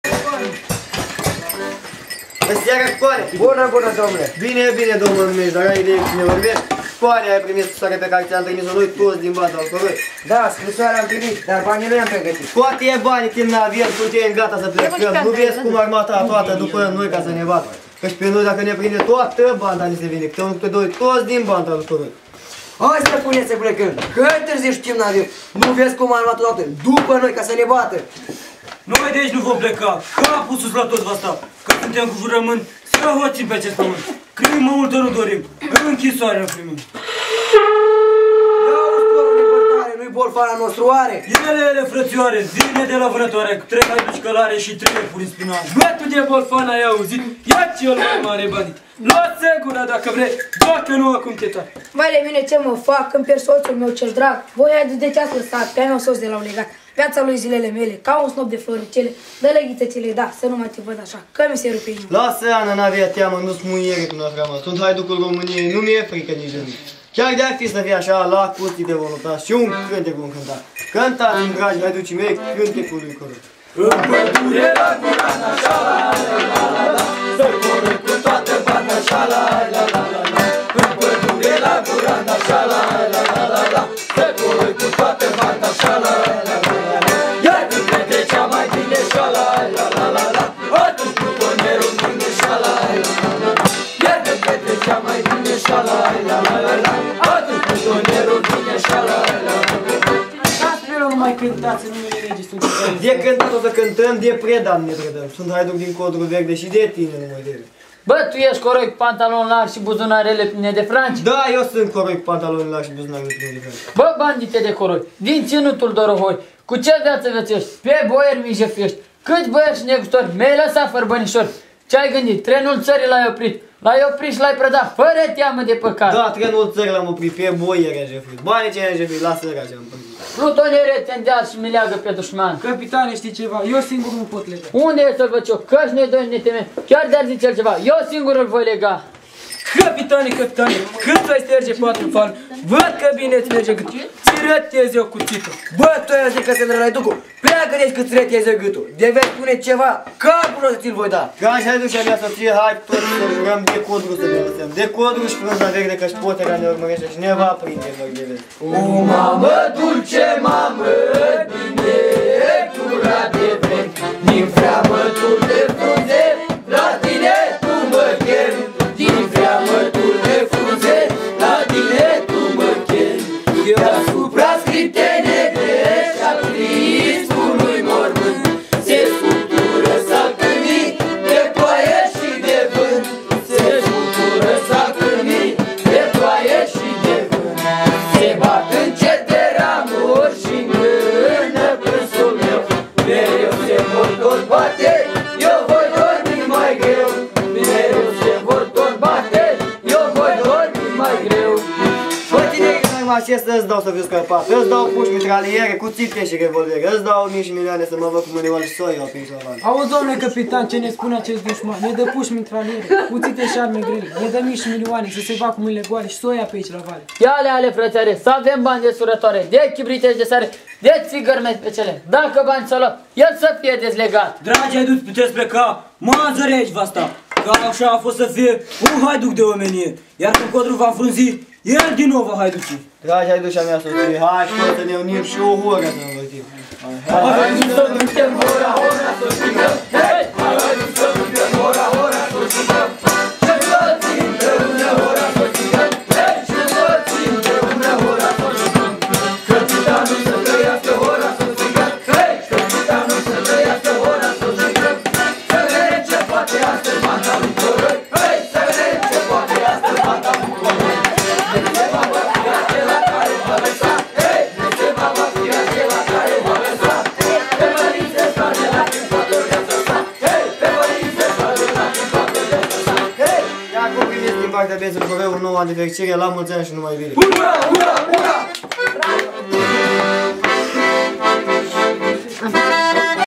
Banda lui Coroi 2011 Todireni
Inregistrare facuta la Todireni-Botosani in noaptea de revelion 2011-Banda lui Coroi